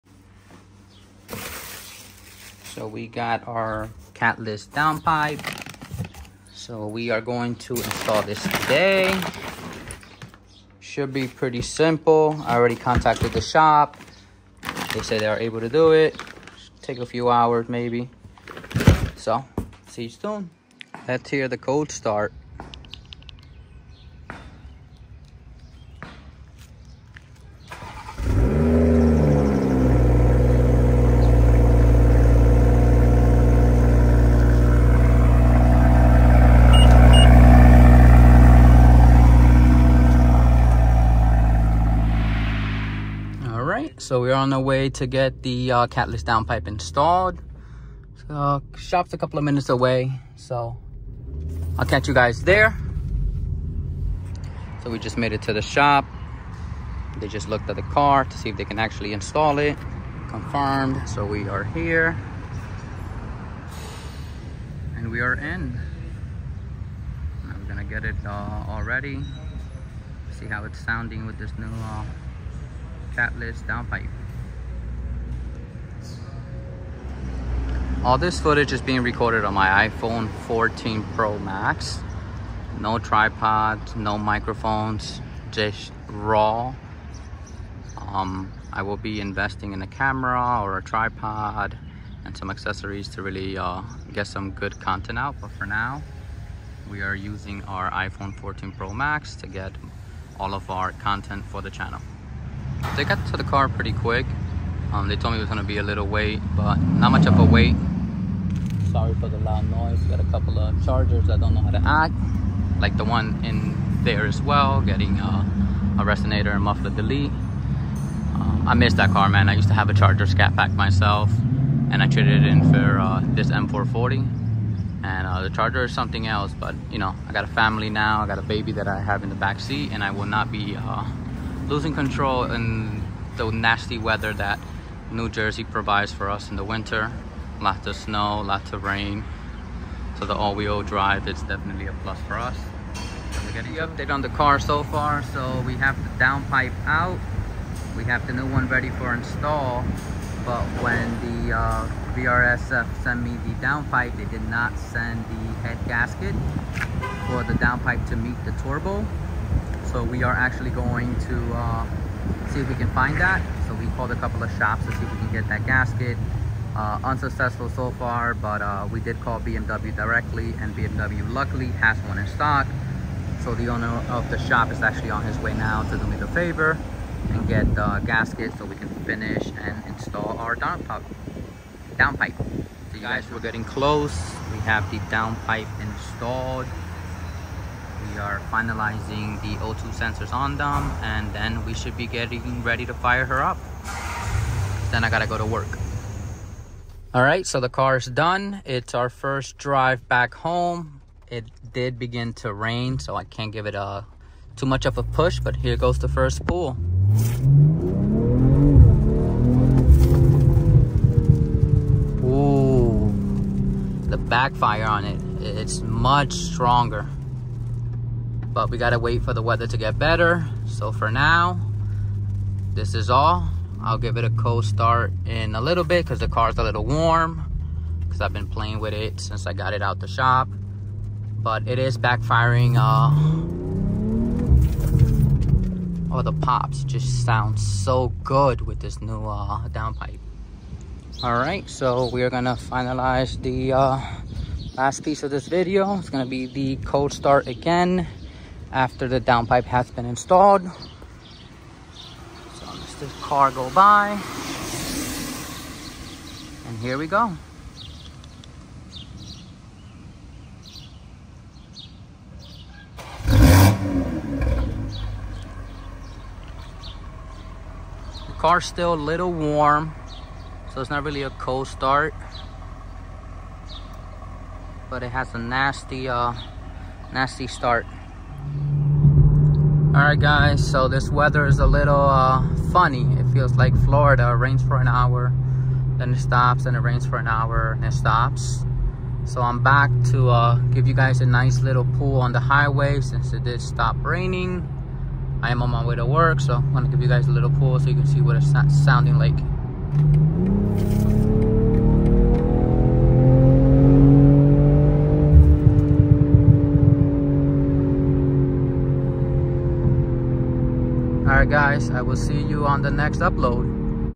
VRSF CATLESS DOWNPIPE best bang for your buck!
VRSF catless downpipe installation on my 2023 G22 M440i.

Anqelrudeeboy